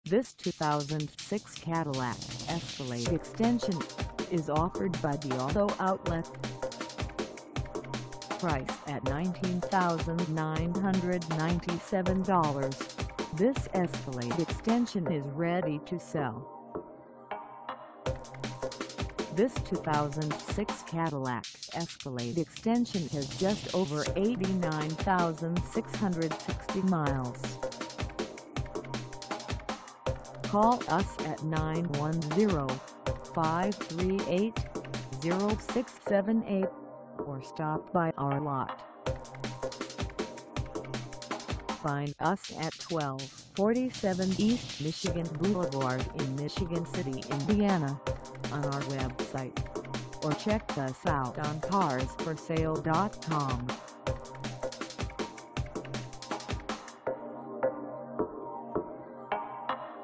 2006 Cadillac Escalade EXT Sport Utility Truck - for sale in
The Auto Outlet
1247 East Michigan Boulevard
in Michigan City, IN 46360

Come test drive this 2006 Cadillac Escalade EXT Sport Utility Truck for sale in Michigan City, IN.

All electrical and optional equipment on this vehicle have been checked and are in perfect working condition. This vehicle has no defects. There is not a door ding on this vehicle. This vehicle is in good running condition. The condition of exterior is extra clean. This vehicle appears to have been garaged. Vehicle has never been smoked in. The interior is in extra clean condition. The mileage represented on this vehicle is the actual current mileage. Great, smooth shifting transmission. There is no previous collision damage to this vehicle. This is your chance to be the second owner of this vehicle. The tires are in good shape and have about 75% of the tread life left.